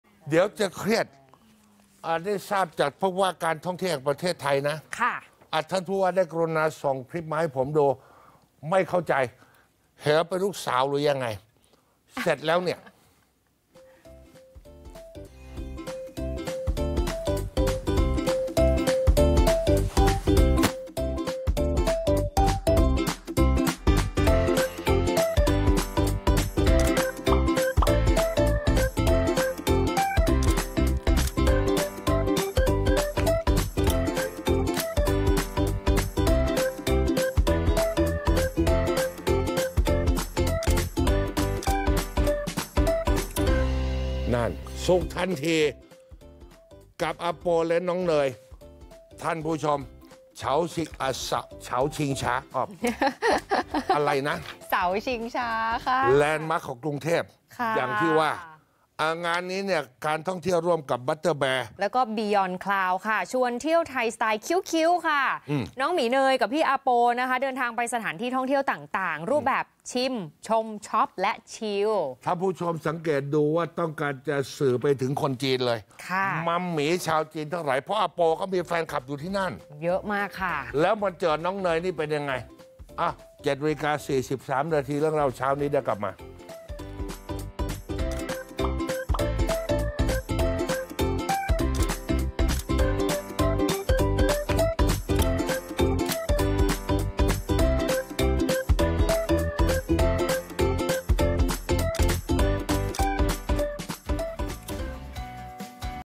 ททท.ดึง 'น้องหมีเนย-อาโป' โปรโมทท่องเที่ยว ดึงมัมหมีจีน เที่ยวไทยแบบคิ้วต์ๆ
ททท. ดึงน้องหมีเนย และอาโป โชว์ความสดใส เปิดแคมเปญ “สุขทันทีกับอาโปและน้องเนย” ชวนมัมหมีชาวจีน เที่ยวไทยตามรอยแบบคิ้วต์ๆ

การท่องเที่ยวแห่งประเทศไทย (ททท.) ร่วมกับ Butterbear และ BeOnCloud ชวนเที่ยวสุดคิ้วท์ (Cute) สร้างความสุข รอยยิ้ม และเสียงหัวเราะ ให้กับทุกคนในแคมเปญ “สุขทันทีกับอาโปและน้องเนย ”

ชวนเซเลปสาวผู้โด่งดังด้วยบุคลิกแสนซน สุดน่ารักอย่าง “น้องเนย” วัยฉามขวบ (3 ขวบ) ด้วยความสดใส ขี้อ้อน ติดตลก ร่าเริง และเป็นดาวเต้น แถมยังมีความน่ารักนอบน้อม เป็นขวัญใจในโซเชียลมีเดียทั้งในไทยและต่างประเทศในขณะนี้ เรียกเสียงฮือฮาทุกครั้งที่ปรากฏตัว และ ‘อาโป-ณัฐวิญญ์ วัฒนกิติพัฒน์’ นักแสดงและนายแบบ ที่มีผลงานร่วมกับแบรนด์ดังระดับโลก มีกลุ่มผู้ติดตามมากกว่า 4 ล้านคนจากหลายประเทศ ทั้ง 2 จะควงแขนกันชวนเที่ยวไทย เชิญชวนนักท่องเที่ยวต่างชาติโดยเฉพาะชาวจีน ร่วมเดินทางท่องเที่ยวตามรอย